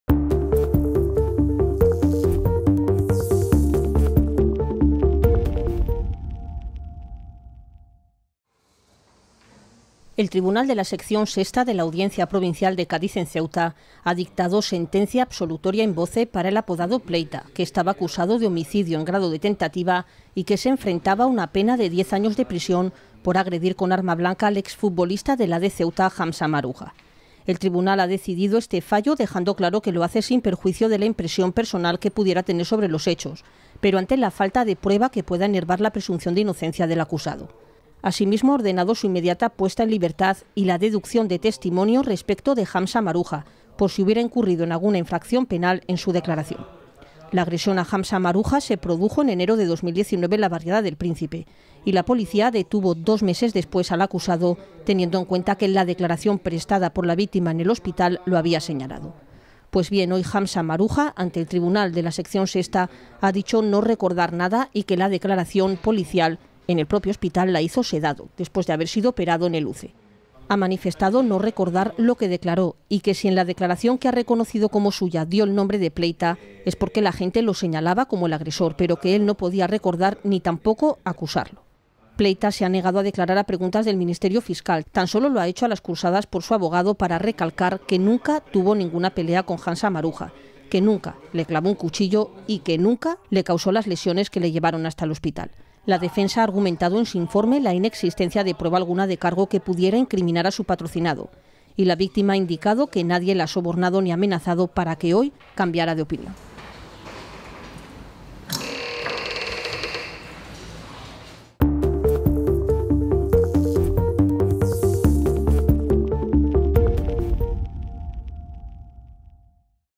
La Audiencia absuelve a ‘Pleita’ de la agresión a Hamza ‘Maruja’
El Tribunal de la Sección VI de la Audiencia provincial de Cádiz en Ceuta ha dictado sentencia absolutoria in voce para el apodado ‘Pleita’, que estaba acusado de homicidio en grado de tentativa y que se enfrentaba a una pena de diez años de prisión, por agredir con arma blanca al ex futbolista del AD Ceuta Hamza ‘Maruja’.

Imagen: Cristian Marfil